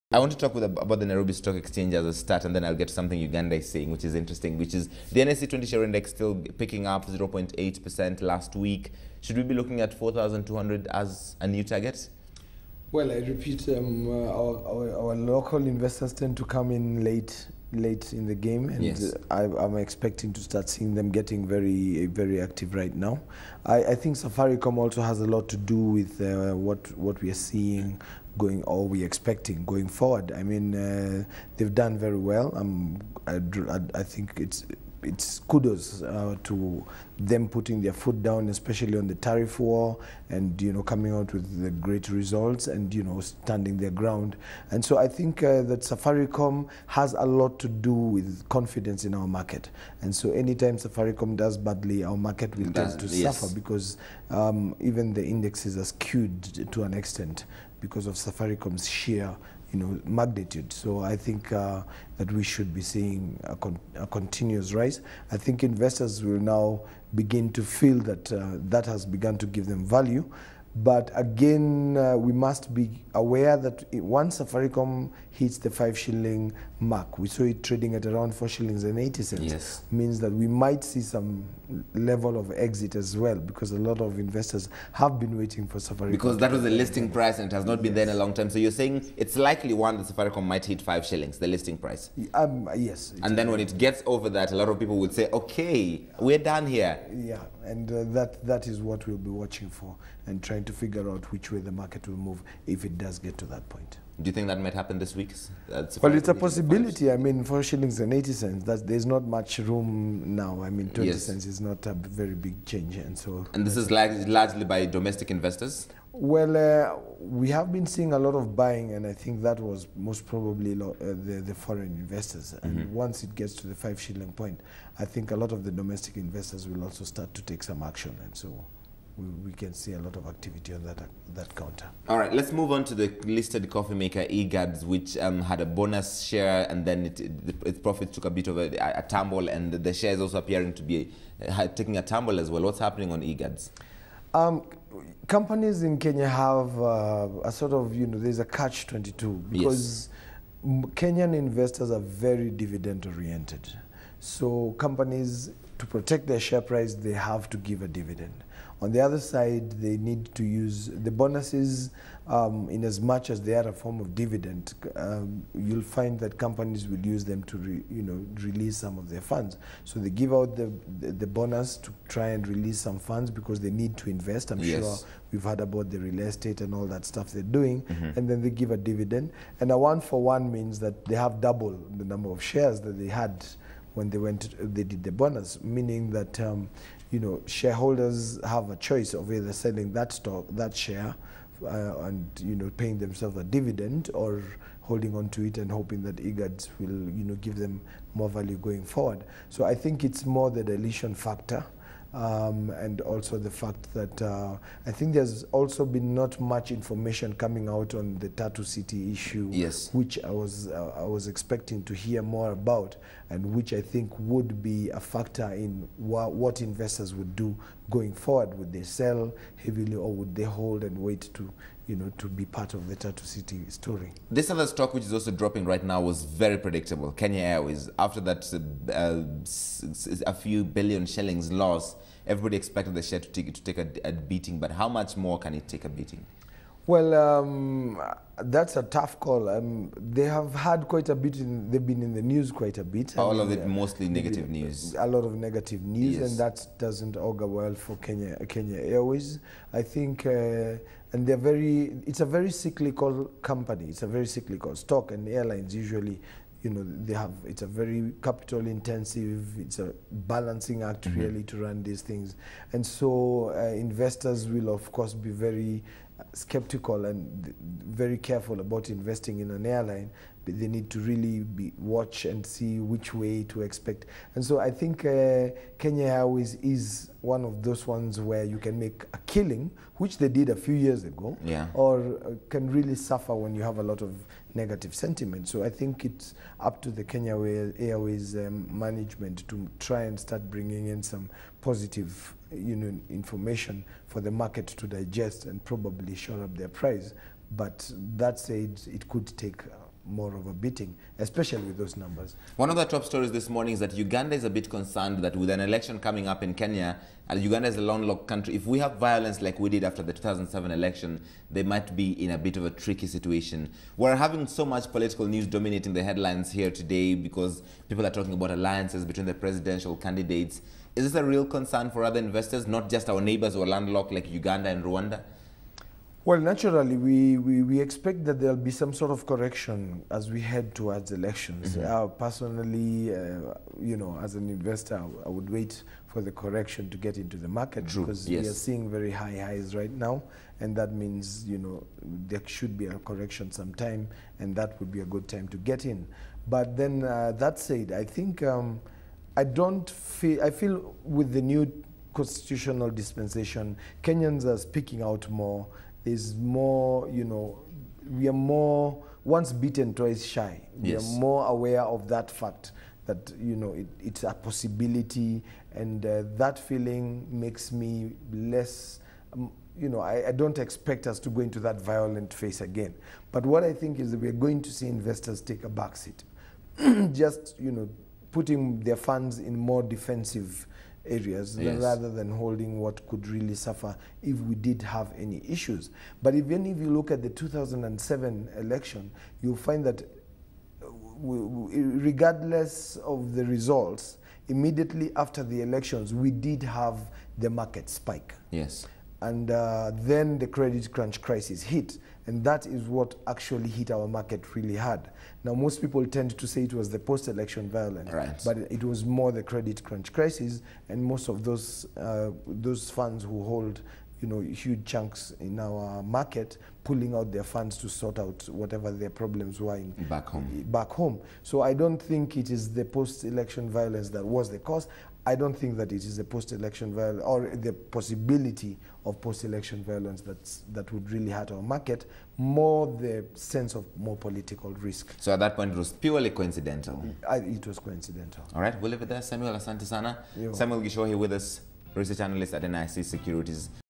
12 November -- Kenya Market Update with Samuel Gichohi
ABN's Larry Madowo speaks with Samuel Gichohi, Research Analyst, NIC Securities, looking at the latest Kenyan markets.